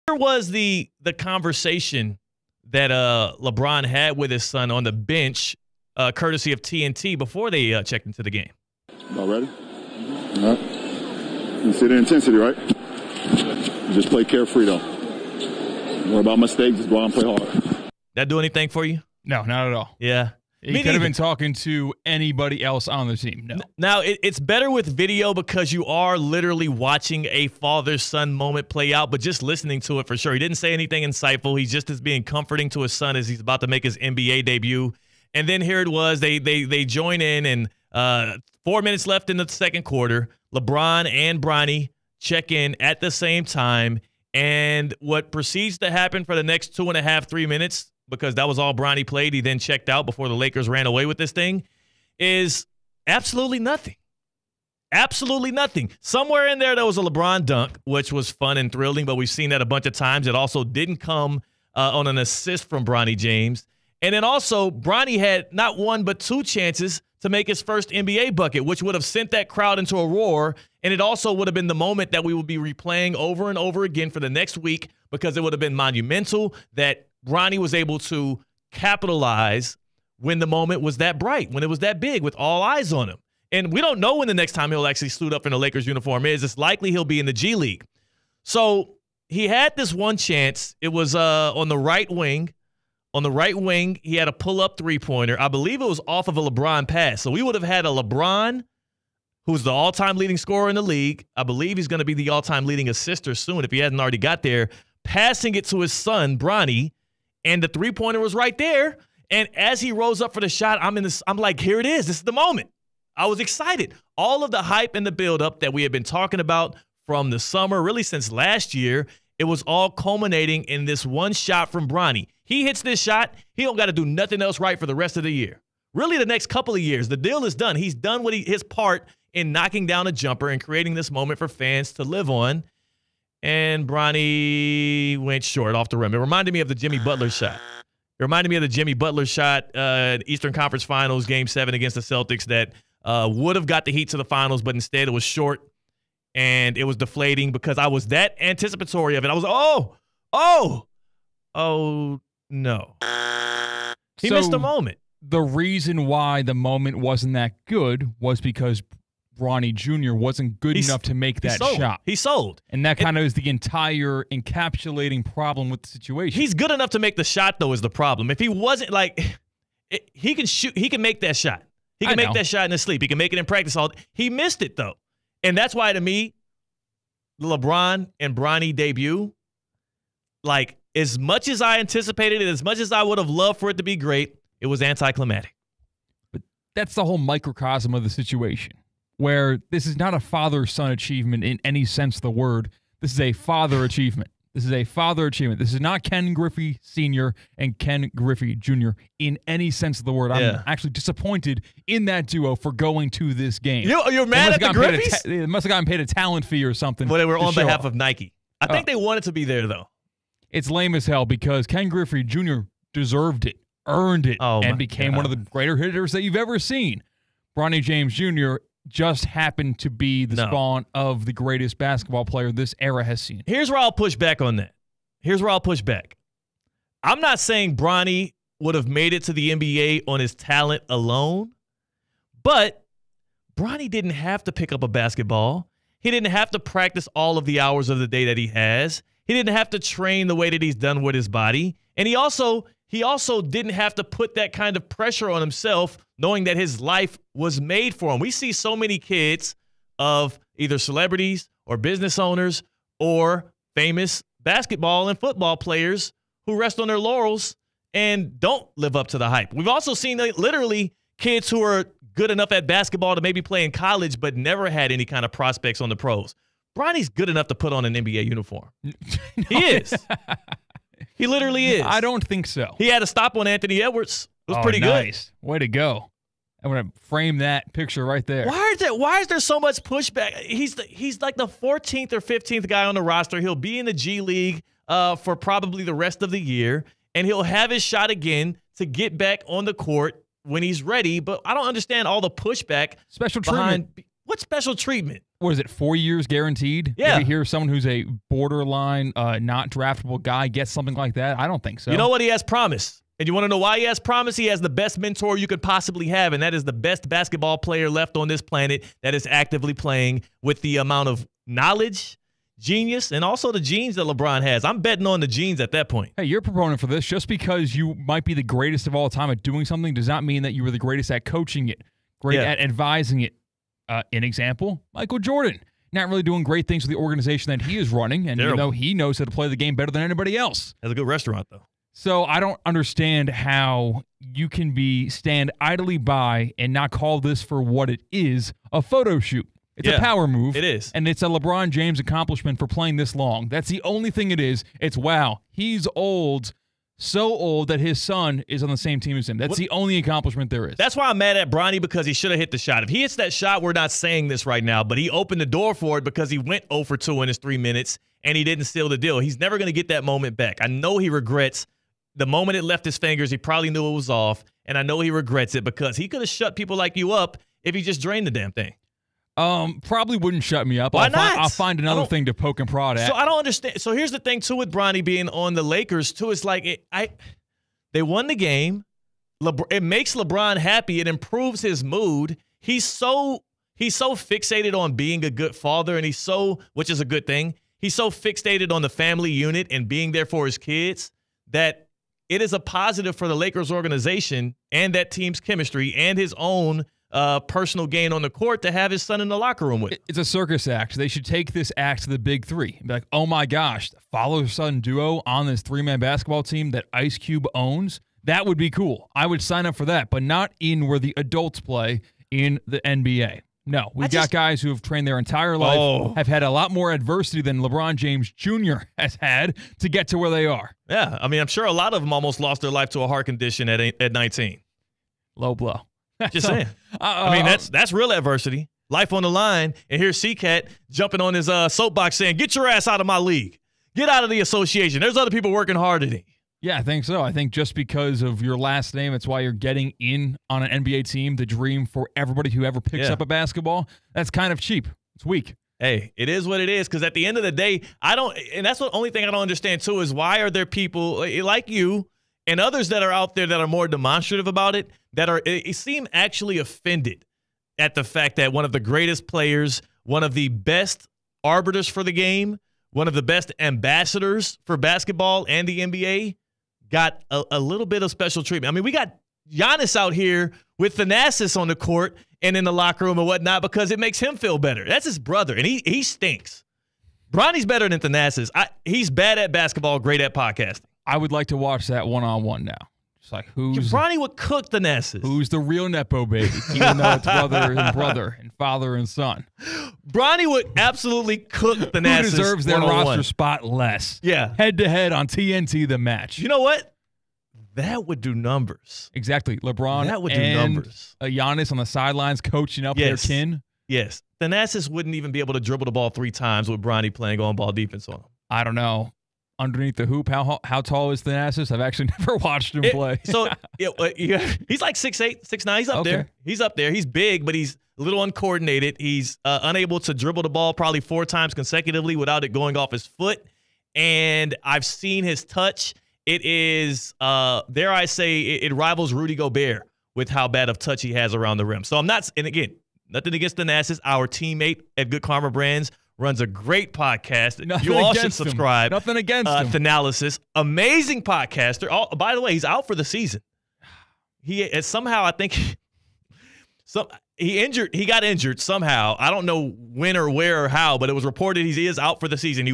LeBron made history, but Bronny missed the mark in Lakers debut
LeBron James and Bronny became the first father-son duo to play in an NBA game together, but in Bronny's 3 minutes of play he didn't deliver a moment to hang onto.